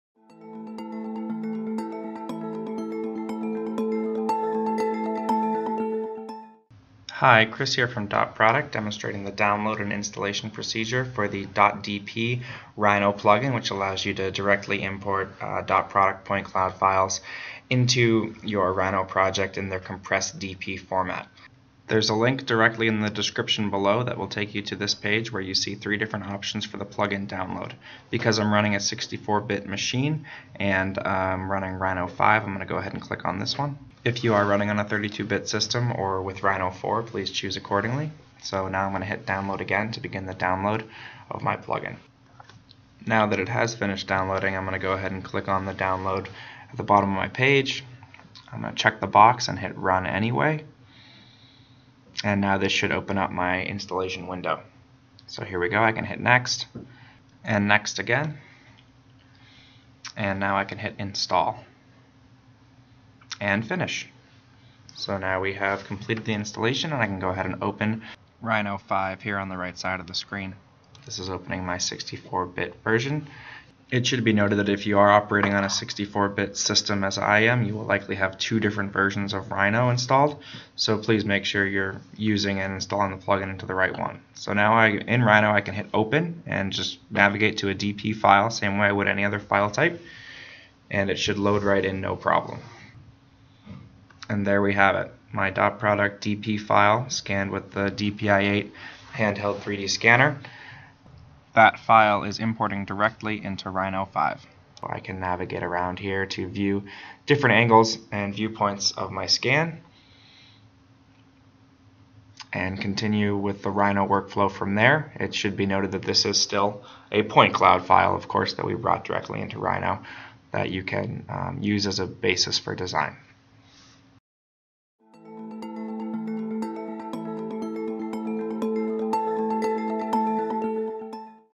Installing the DotProduct Rhino Plugin
A brief tutorial on the installation procedure for the DotProduct plugin for Rhino 4 or 5. This allows direct import of .DP files.